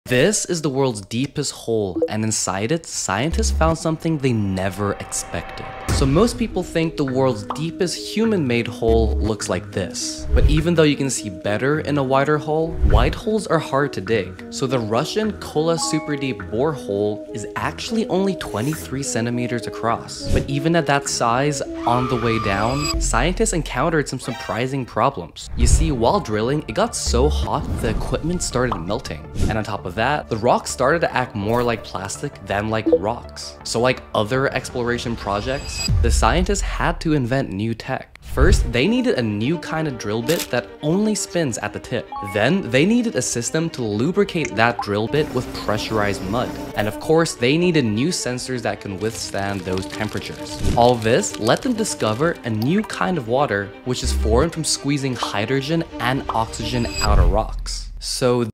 What's Inside the World's Deepest Hole?
This is the World's Deepest Hole. And Inside It, Scientists Found Something They Never Expected.

So most people think the world's deepest human made hole, looks like this. But even though you can see better in a wider hole, wide holes are hard to dig. So the Russian Kola Superdeep Borehole is actually only 23 centimetres across. But even at that size, on the way down,

scientists encountered some surprising problems.

You see, while drilling, it got so hot that the equipment started melting. And at on top of that, the rocks started to act more like plastic than rocks. So like other exploration projects,

the scientists had invent new tech.

First, they needed new kind of drill bit that only spins at the tip. Then they needed a system to lubricate the drill with pressurised mud. And of course they needed new sensors that can withstand those temperatures. All this let them discover a new kind of water which formed from squeezing oxygen and hydrogen out of rocks.

So